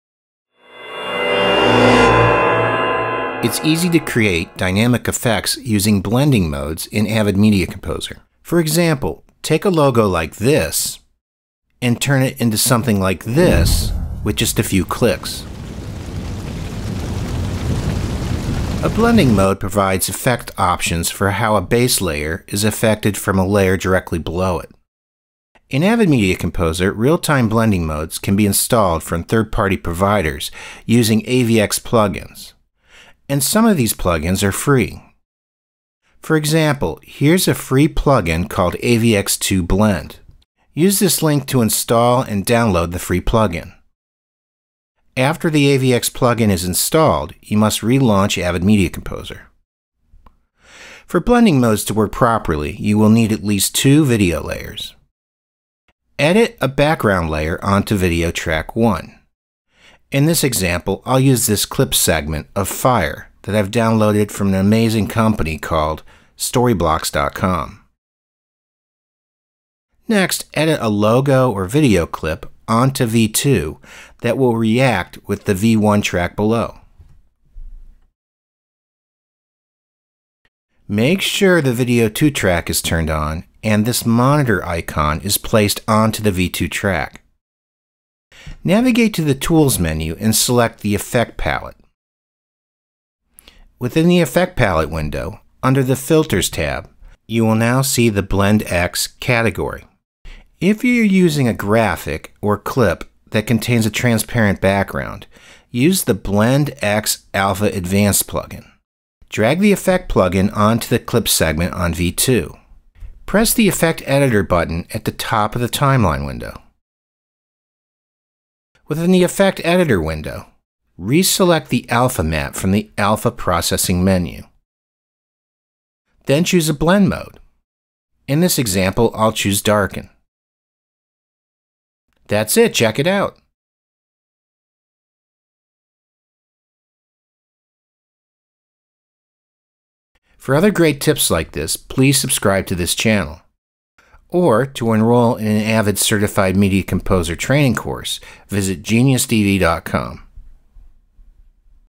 Blending Modes in Avid Media Composer
Learn how to use real-time blending modes for video clips within Avid Media Composer by using FREE AVX Plugins.